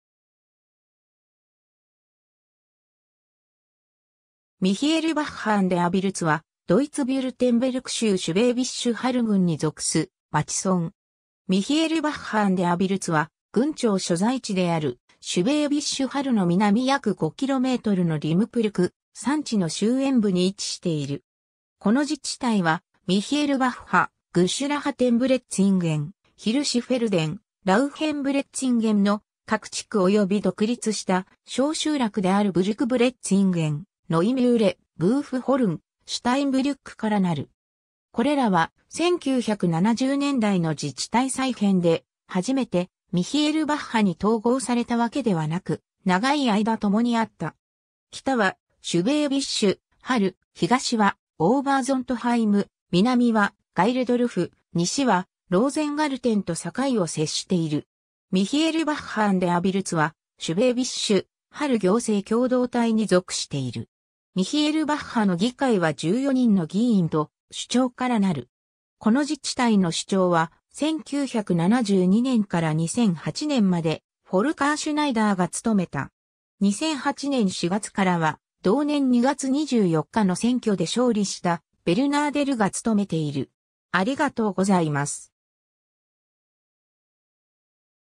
ミヒェルバッハ・アン・デア・ビルツ
シュヴェービッシュ・ハル郡
ミヒェルバッハ・アン・デア・ビルツ (ドイツ語: Michelbach an der Bilz) は、ドイツ連邦共和国バーデン＝ヴュルテンベルク州シュヴェービッシュ・ハル郡に属す町村（以下、本項では便宜上「町」と記述する）。
 ミヒェルバッハ・アン・デア・ビルツは、郡庁所在地であるシュヴェービッシュ・ハルの南約5kmのリムプルク山地の周縁部に位置している。
 この自治体は、ミヒェルバッハ、グシュラハテンブレッツィンゲン、ヒルシュフェルデン、ラウヘンブレッツィンゲンの各地区および独立した小集落であるブルクブレッツィンゲン、ノイミューレ、ブーフホルン、シュタインブリュックからなる。
これらは、1970年代の自治体再編で初めてミヒェルバッハに統合されたわけではなく、長い間ともにあった。
 北はシュヴェービッシュ・ハル、東はオーバーゾントハイム、南はガイルドルフ、西はローゼンガルテンと境を接している。
 ミヒェルバッハ・アン・デア・ビルツは、シュヴェービッシュ・ハル行政共同体に属している。
 ミヒェルバッハの議会は14人の議員と首長からなる。
 この自治体の首長は、1972年から2008年までフォルカー・シュナイダーが務めた。
2008年4月からは、同年2月24日の選挙で勝利したヴェルナー・デルが務めている。